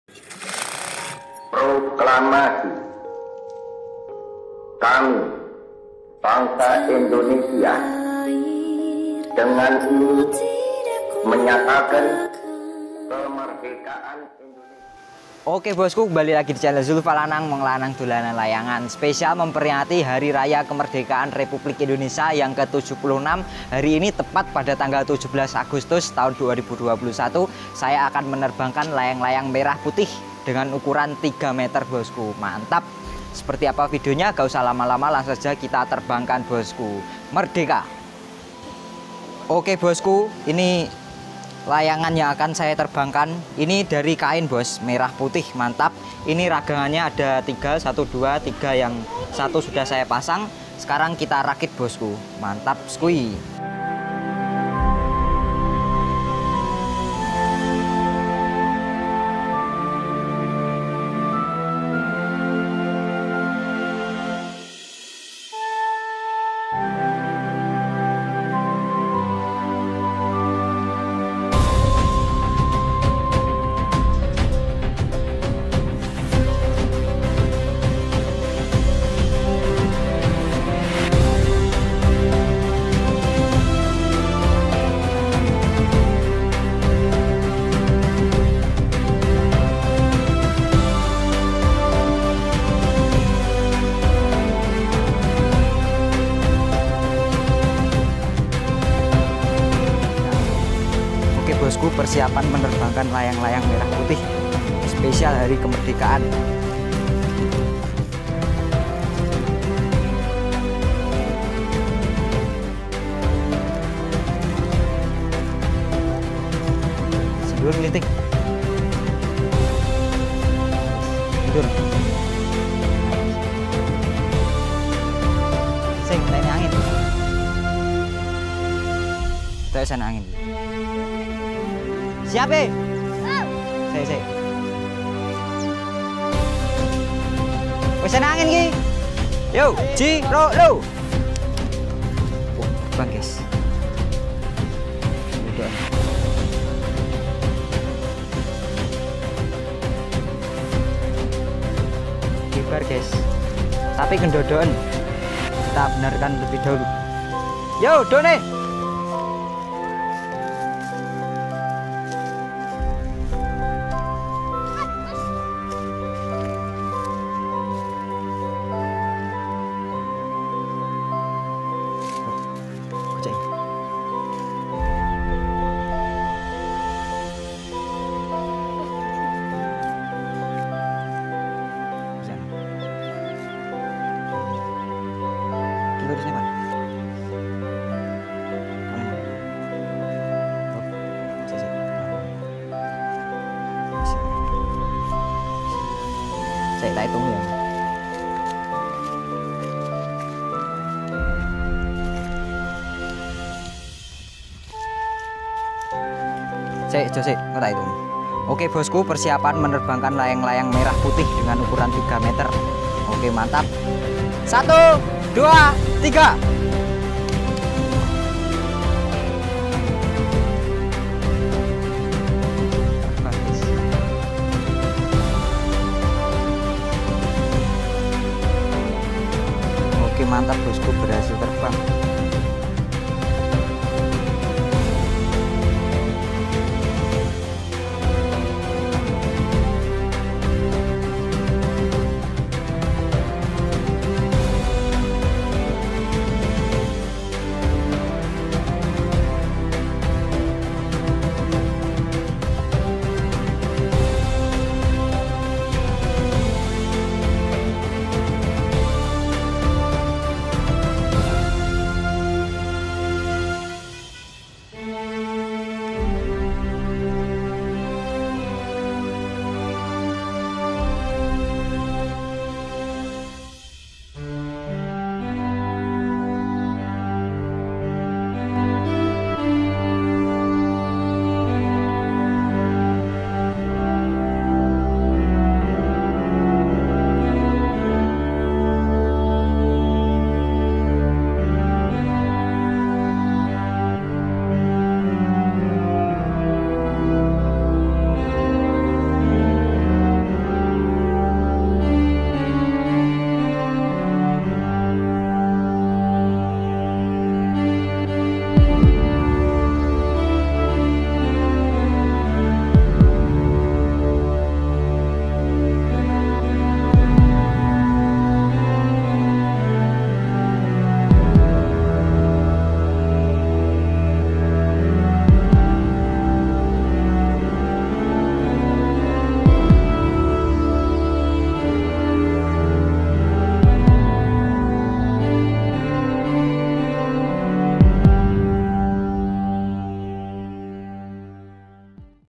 TERBANGKAN LAYANGAN MERAH PUTIH 3 METER !!! HUT RI KE 76 🇲🇨
layangan merah putih yang terbuat dari kain merah dan putih yang di jahit, Layangan merah putih terbang berkibar di langit indonesia pada selasa 17 Agustus 2021, memperingati hari kemerdekaan republik indonesia yang ke 26, layangan dirgahayu republik indonesia sukses berkibar di langit indonesia.